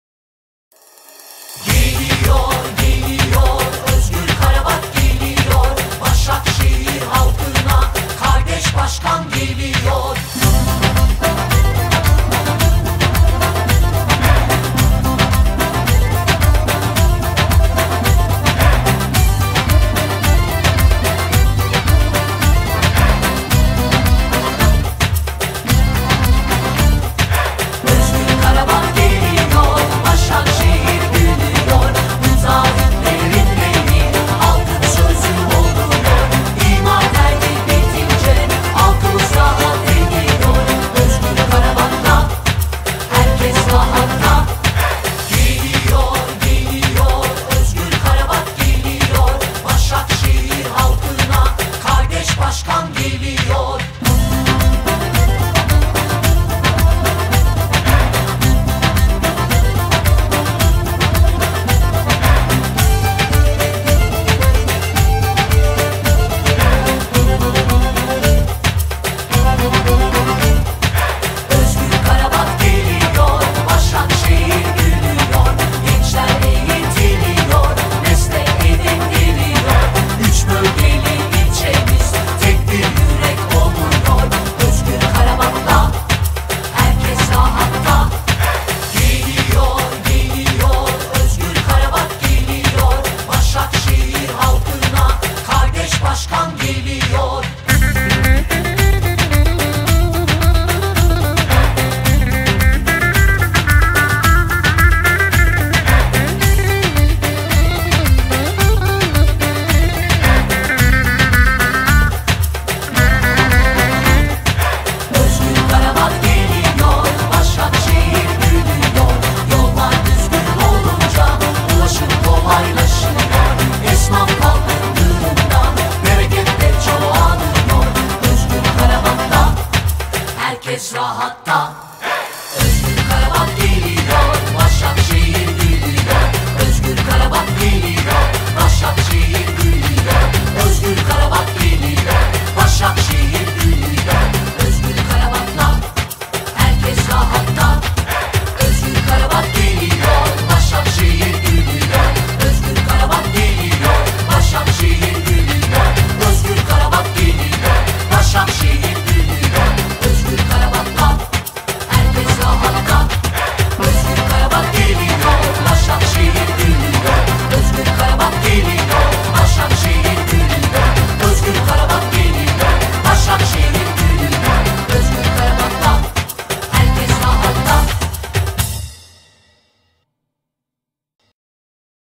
Şahintepe Mahallesi Yürüyüşümüz
23 Şubat Pazar günü gerçekleştirdiğimiz Şahintepe Mahallesi yürüyüşümüzde yurttaşlarımızın, komşularımızın ve hemşehrilerimizin büyük ilgisiyle karşılandık.

CHP Başakşehir Belediye Başkan Adayı Özgür Karabat'ın resmi YouTube kanalıdır.

Daha Fazlası için adresini ziyaret edebilirsiniz.